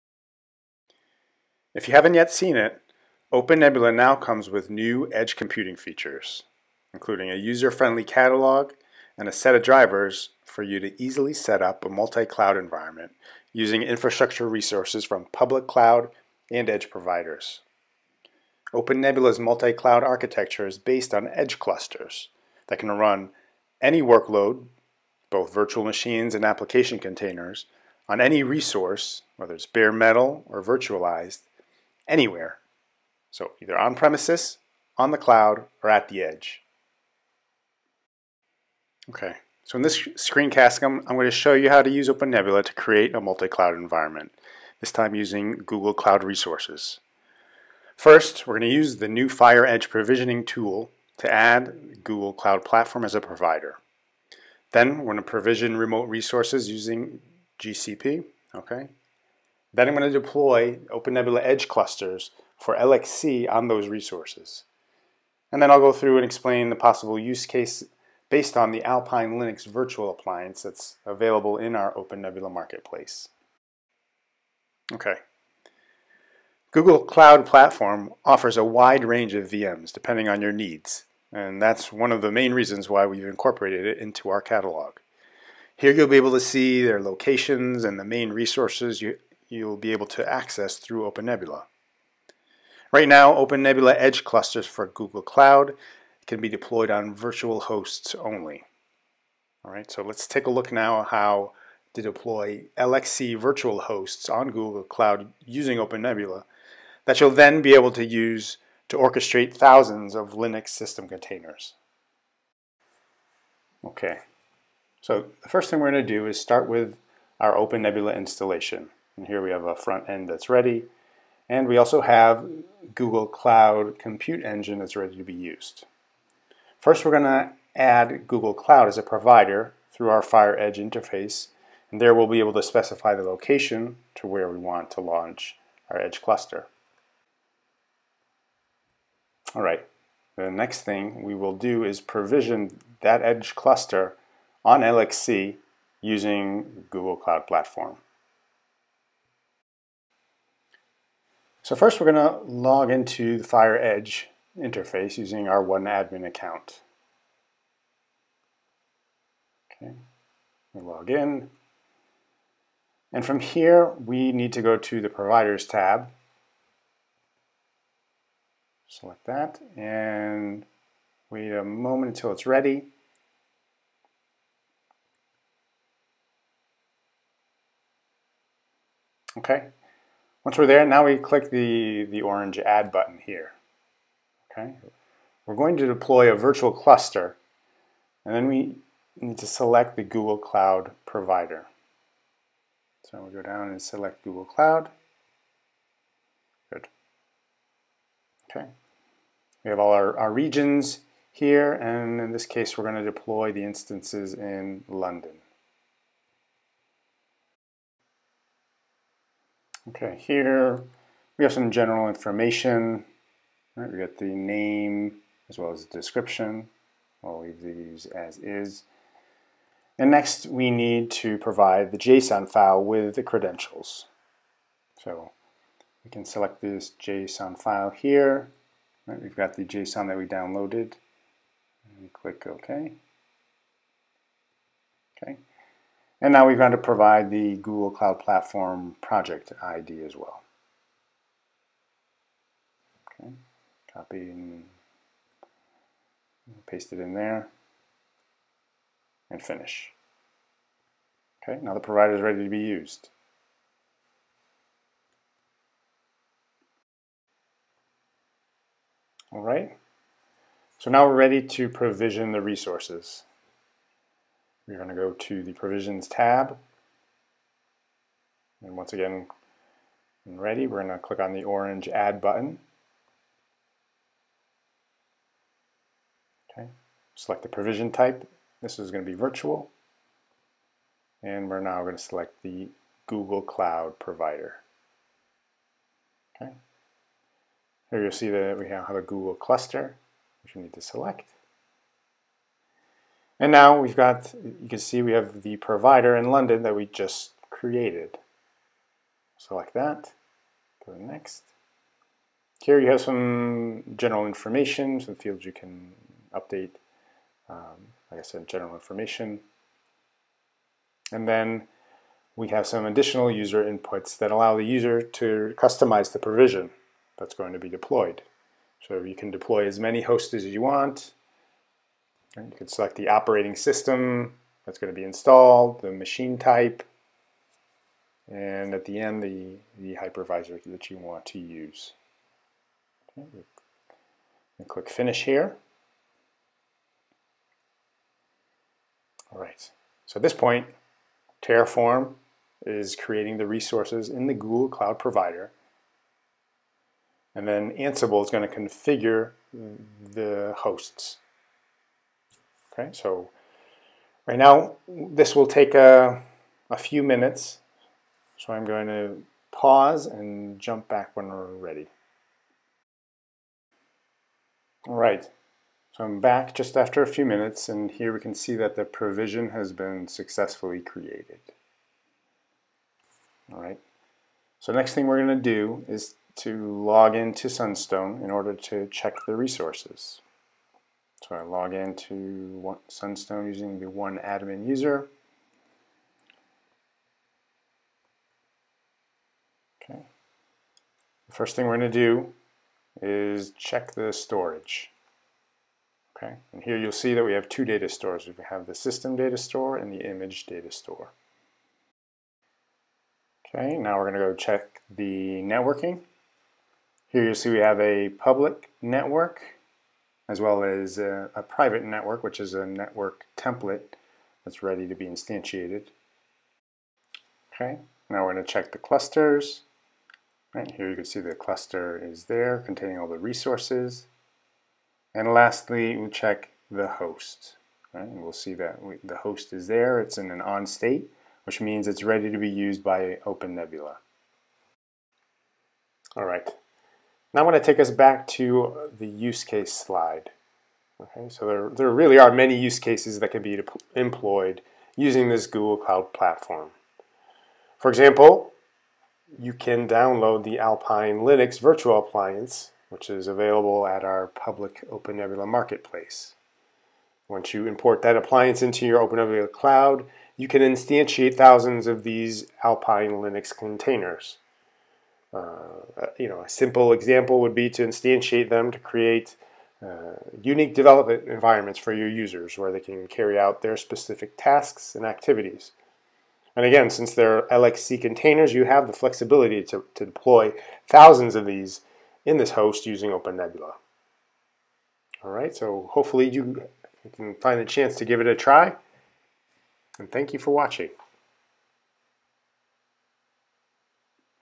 OpenNebula - Expanding Your OpenNebula Multi-Cloud with Google Cloud
OpenNebula now comes with new edge computing features, including a user-friendly catalog and a set of drivers for you to easily set up a multi-cloud environment using infrastructure resources from public cloud and edge providers. This screencast shows how easy it is now to use OpenNebula, and its new FireEdge GUI, to provision Google Cloud resources to create a multi-cloud environment, in this case for the deployment of LXC system containers.

🇪🇺 This work has received funding from the EU Horizon 2020 research and innovation programme under grant agreement ONEedge 880412.